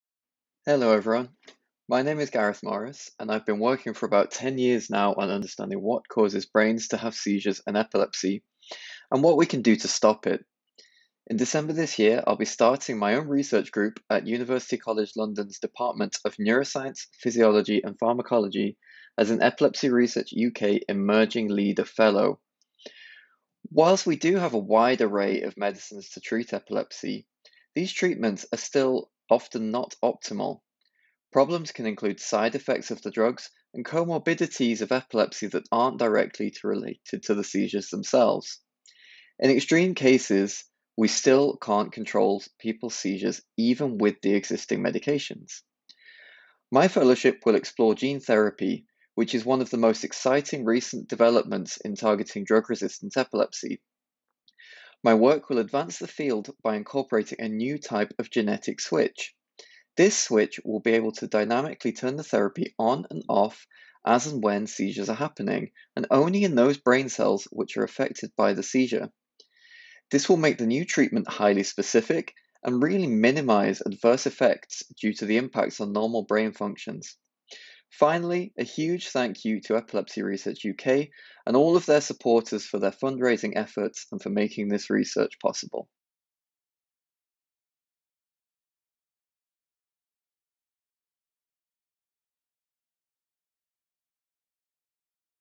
Dr Gareth Morris - Emerging Leader Fellowship Award
Title: miRsense - Using microRNA biosignatures as sensors for precision gene therapy
Institute: University College London
Supervisors: Professor Stephanie Schorge (University College London)

The Study
I will learn exactly which types of brain cell are active in seizures, and how the molecules inside these cells change in epilepsy. Based on this, I will design a gene therapy that is only switched on when these molecules are present during seizures. The rest of the time it will be switched off and do nothing to the brain. This new technique could hugely refine how we treat temporal lobe epilepsy and other drug-resistant epilepsies. It could represent a ‘one-off’ treatment, and may mean that people with epilepsy no longer require medications to control their seizures.
Impact of this research
I hope to complete initial pre-clinical testing within the 3 years of this fellowship. The host institute is already leading clinical trials of similar treatments, so we would have the infrastructure to move the new treatment directly to clinical trial in the medium term.